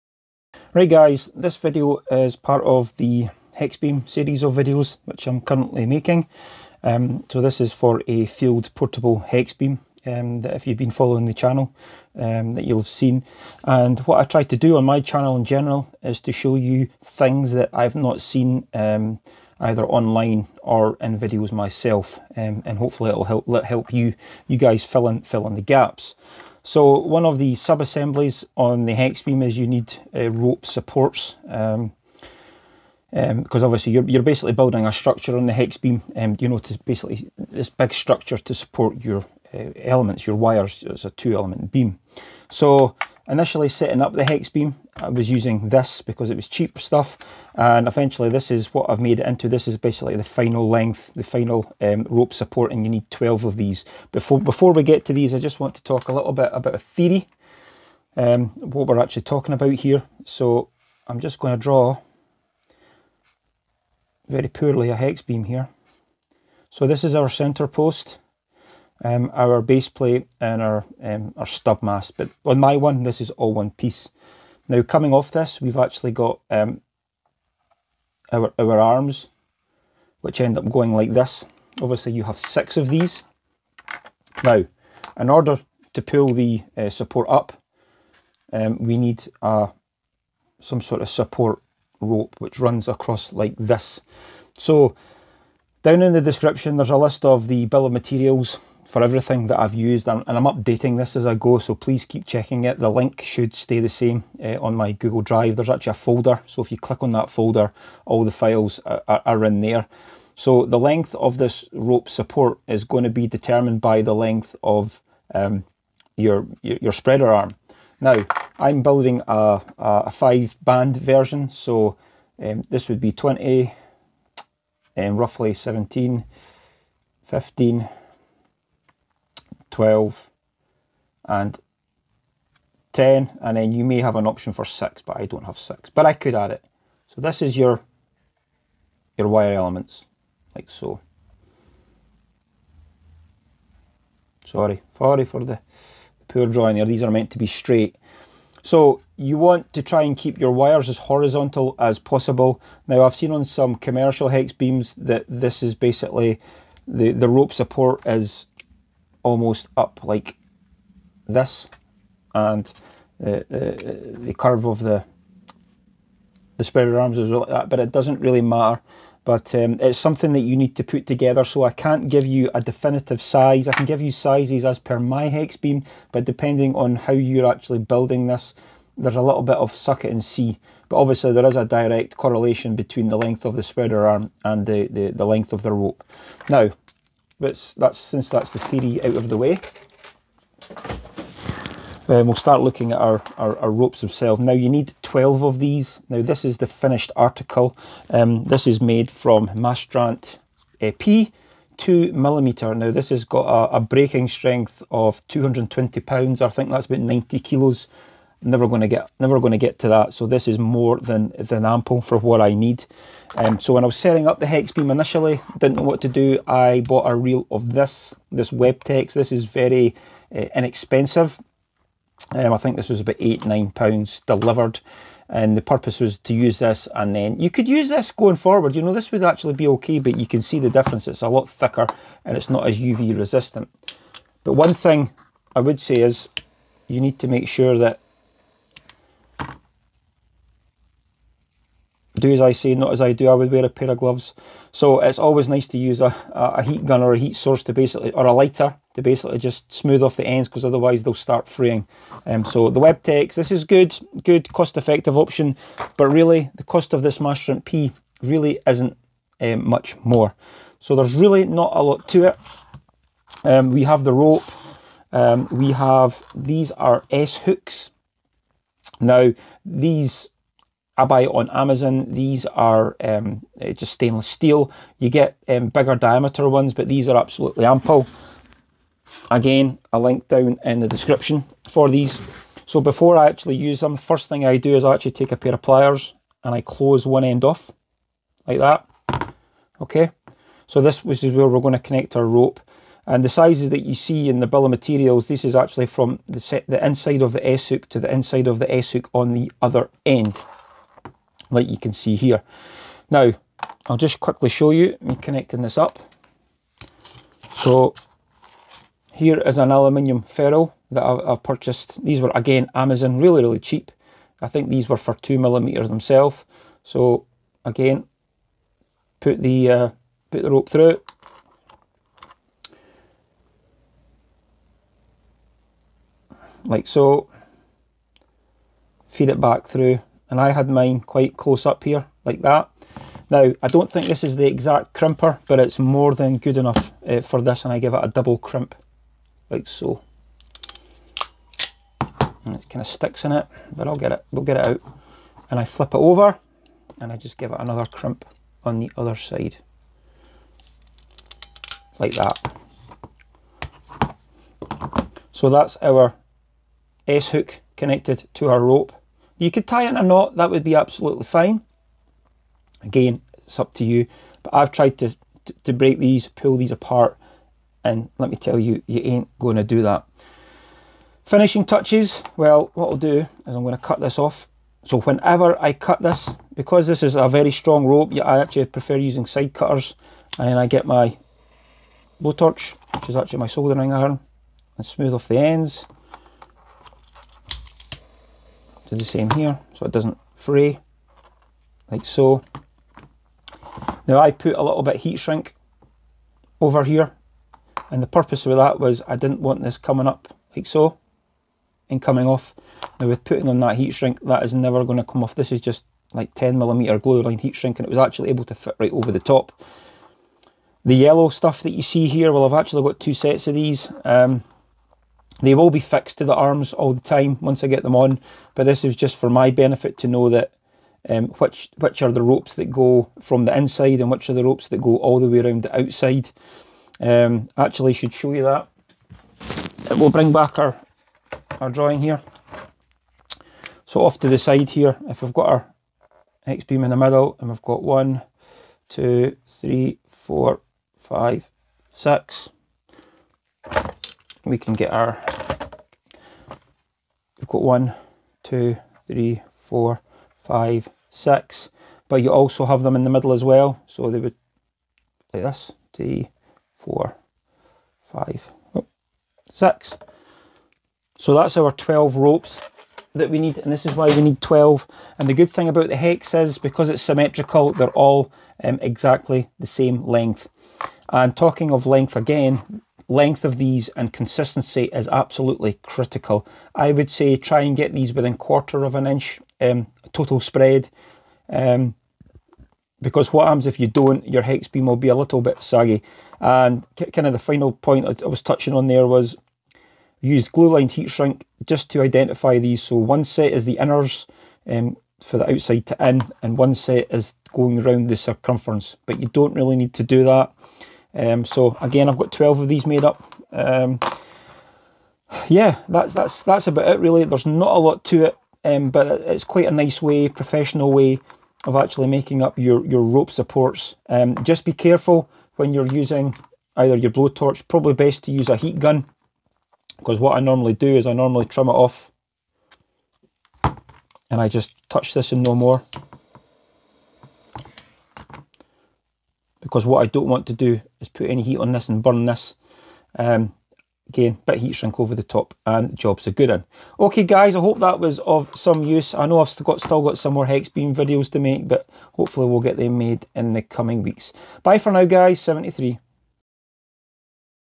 How to Make Professional Looking Hexbeam Rope Sets
How to make Rope Supports for your Hexbeam.

Link to my Hexbeam files - Note its still a work in progress so please check back often.